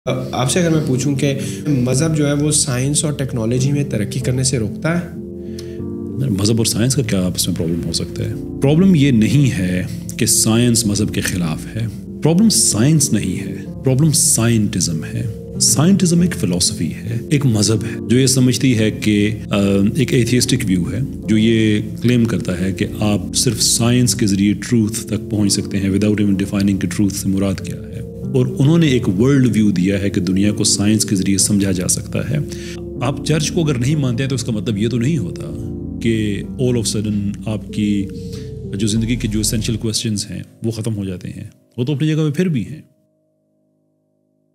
Scientism vs Religion | ft. Qaiser Ahmed Raja
In this episode of NAEEM_SIKANDAR_PODCAST, we have invited Mr. @QaiserAhmedRaja who is a business development expert. Qaiser has helped many corporate businesses and taken them to the next level but he is known for opposing liberal ideas on social media. in this podcast, I talked to Mr. Raja about atheism, liberalism, and trans rights through the lens of Islam. I asked frequently asked questions about religion on the internet from Qaiser.